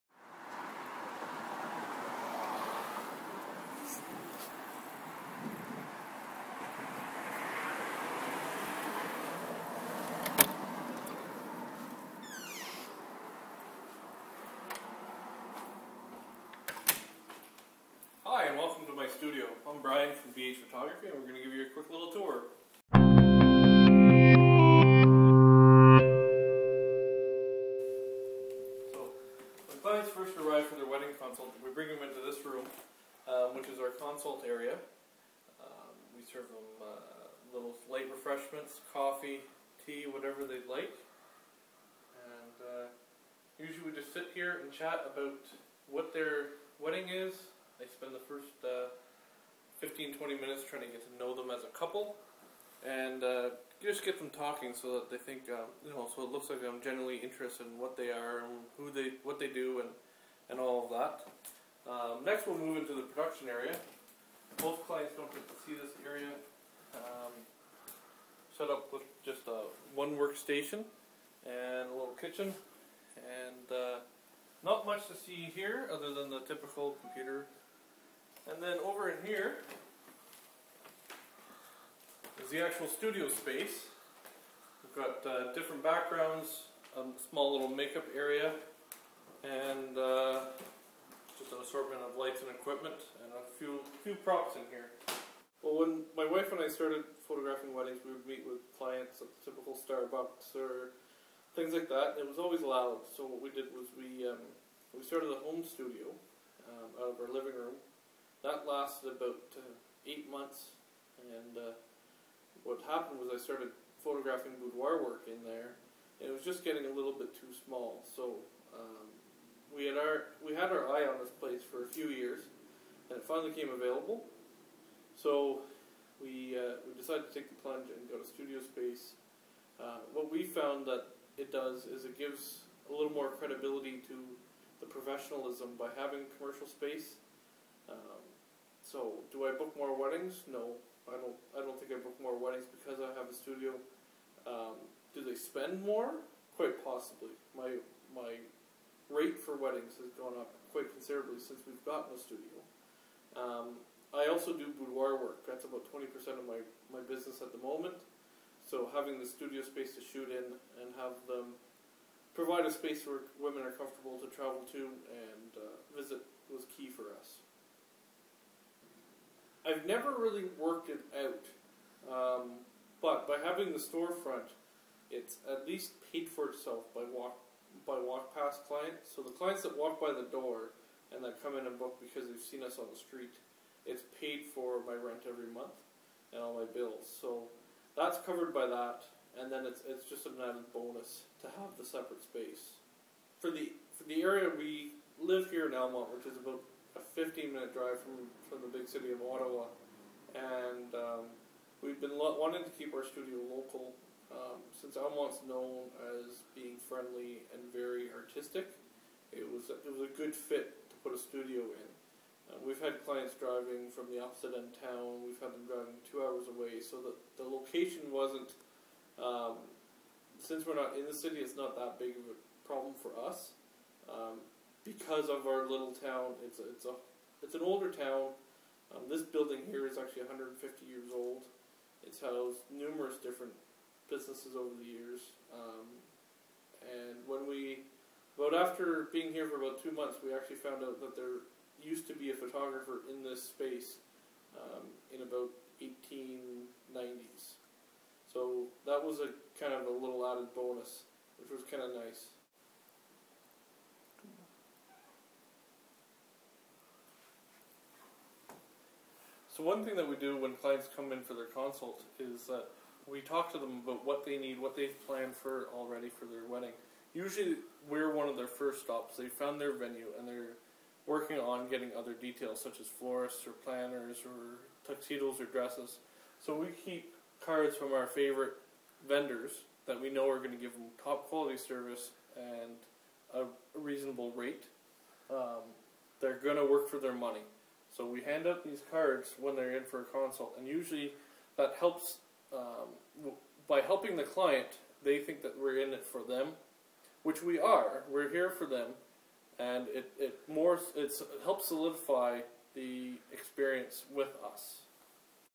Cribs: Brian Hargreaves
Here's a quick tour through Brian Hargreaves' studio in Alamonte, Ontario, a small town outside of Canada's capital city, Ottawa.